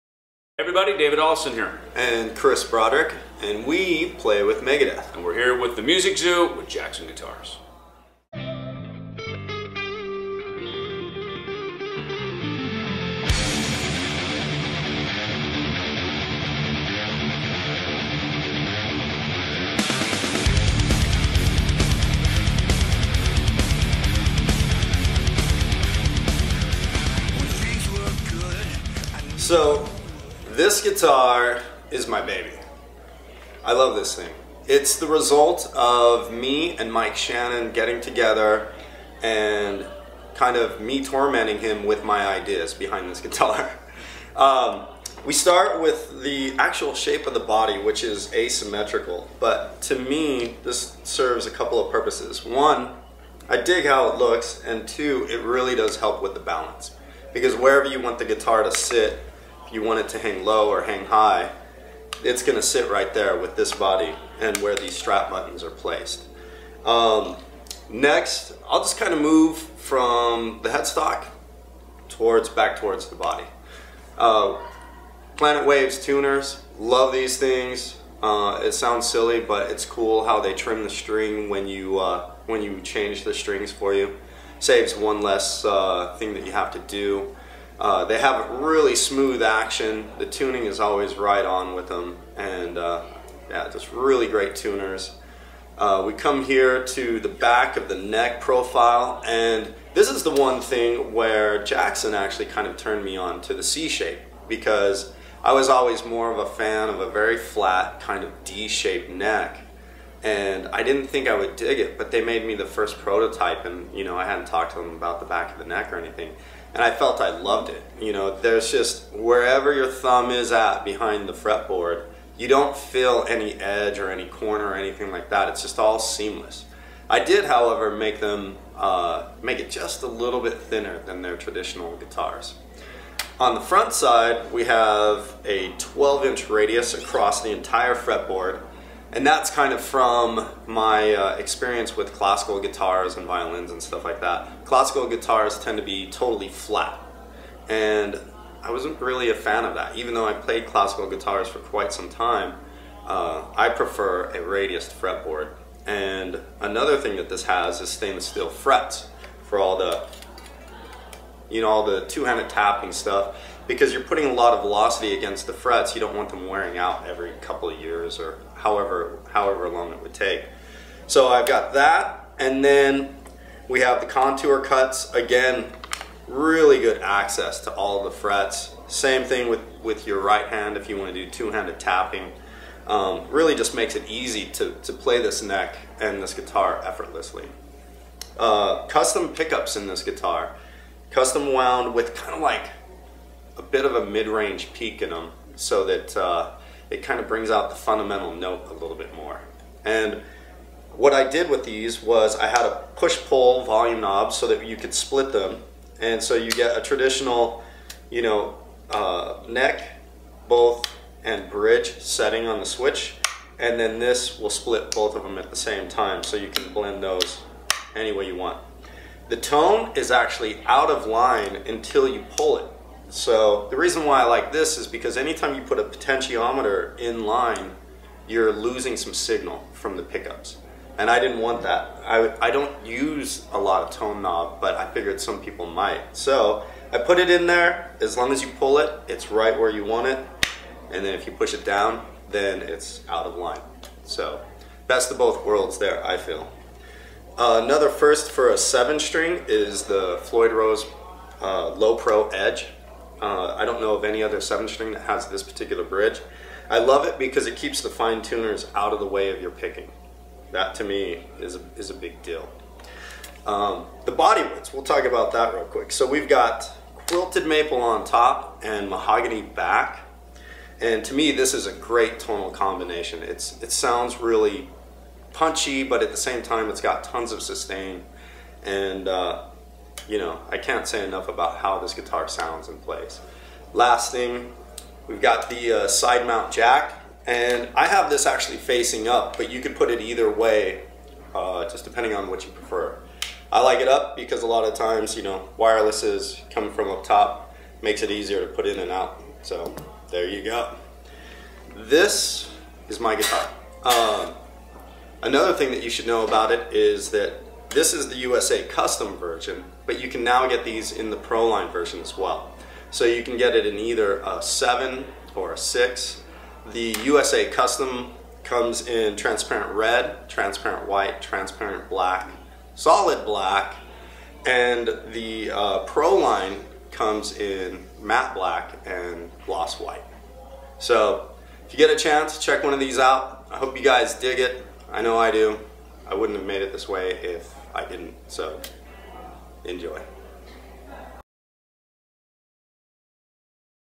Chris Broderick and Dave Ellefson of Megadeth Talk About Their Signature Jackson Instruments Part I
In November of 2014, on behalf of Jackson Guitars, Dave Ellefson of Megadeth stopped by The Music Zoo showroom before their clinic later that night to talk about their signature Jackson instruments. In this video, Chris Broderick talks about his custom and pro series line of guitars with Jackson.